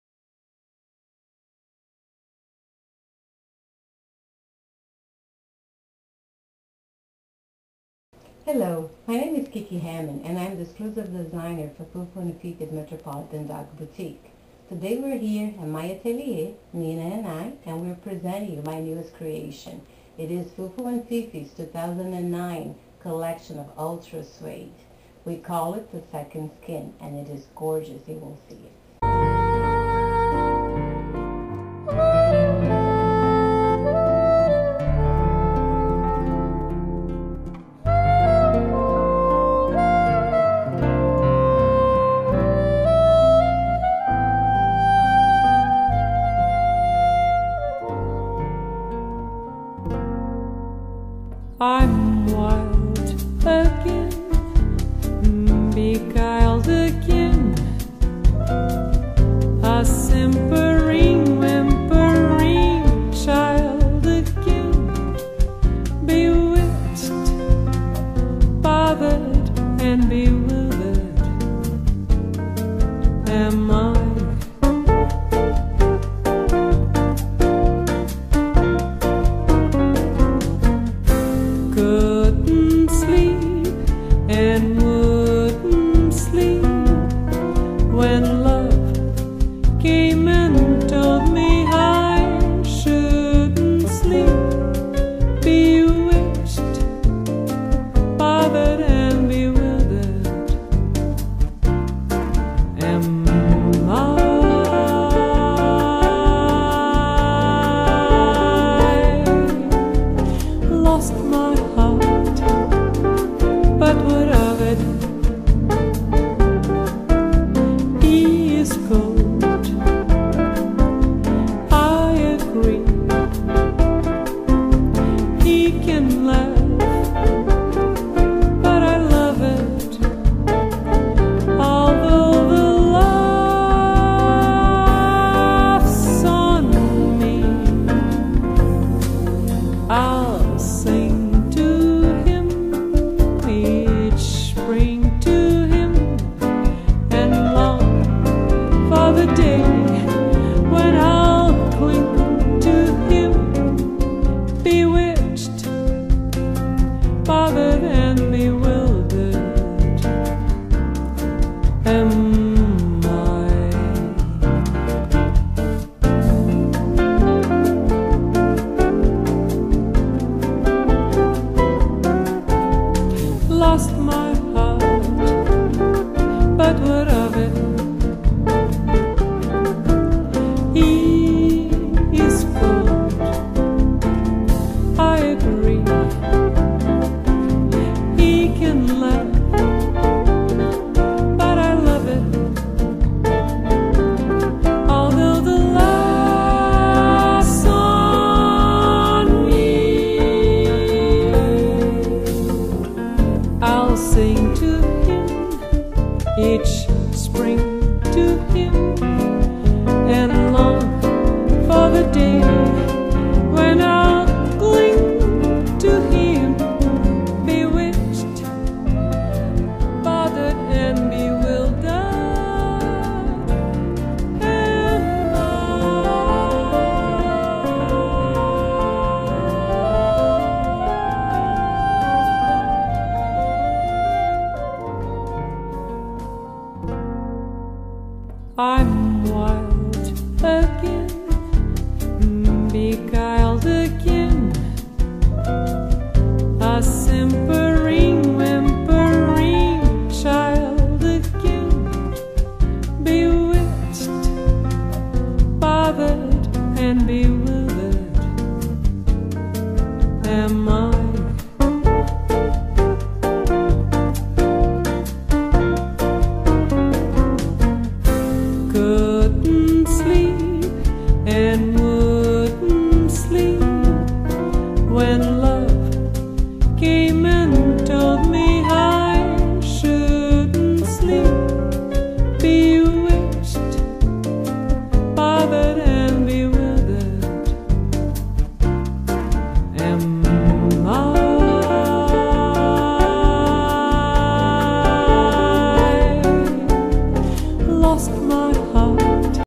Fufu & Fifi Ultra Suede Skin line 2009 Catalog- 0001
The presentation of Fufu&Fifi's 2009 Winter catalog
Unique Pcs one of a kind only! Customized Couture